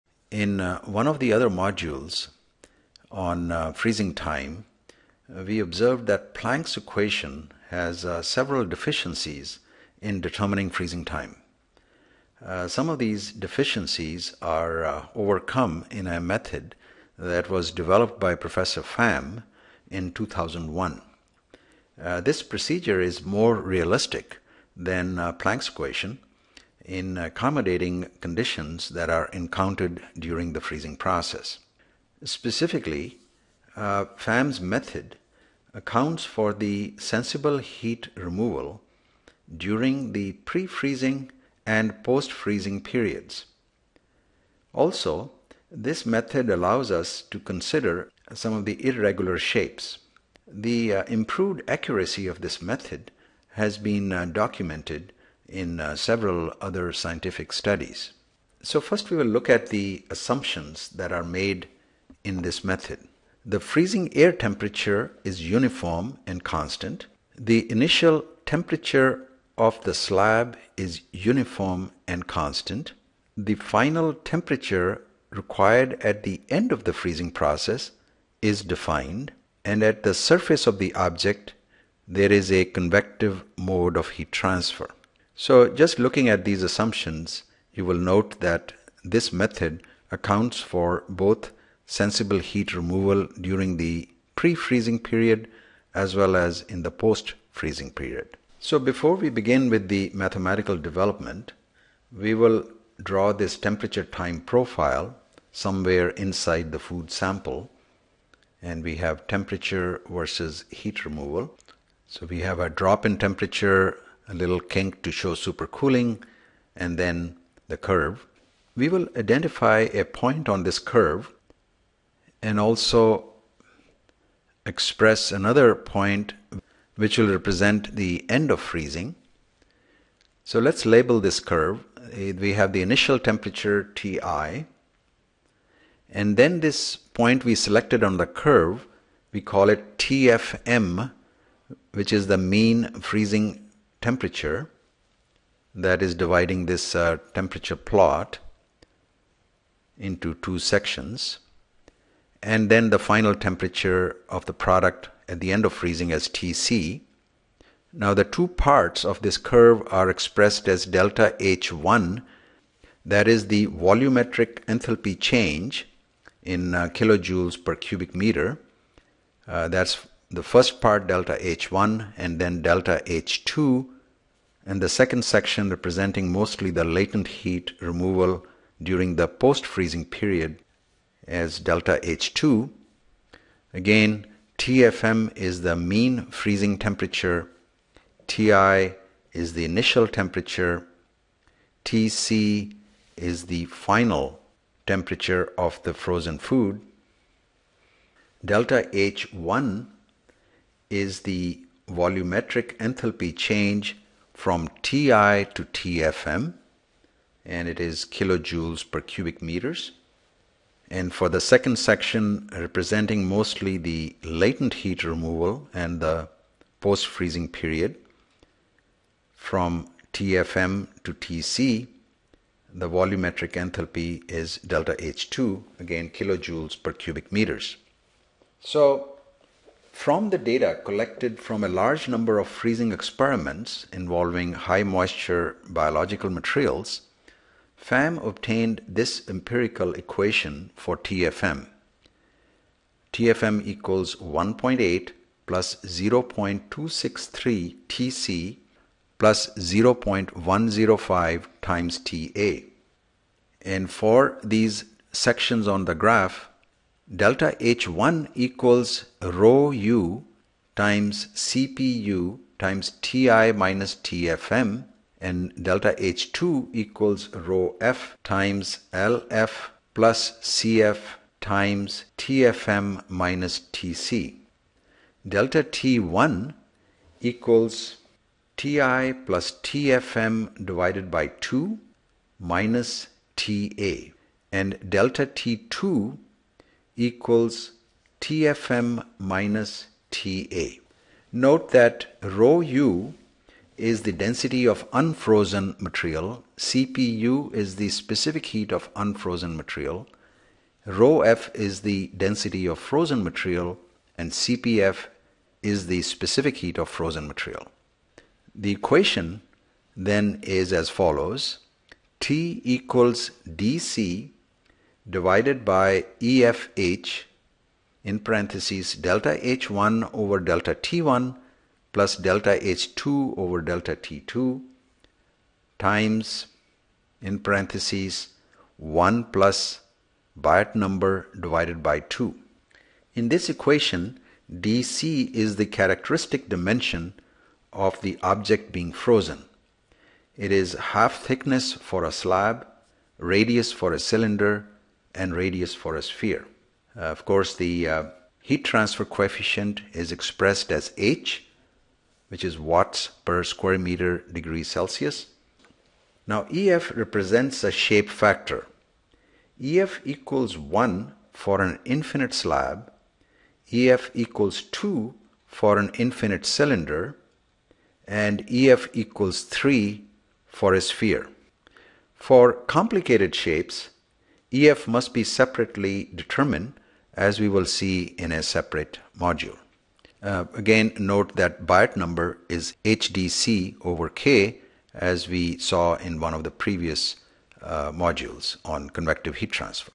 Food Freezing - Estimate freezing time using Pham's Method
Derivation of expression used for estimating freezing time using Pham's method.
Please provide feedback on this module by selecting "Like" or "Dislike". Your feedback and comments is important to me in developing new tutorials.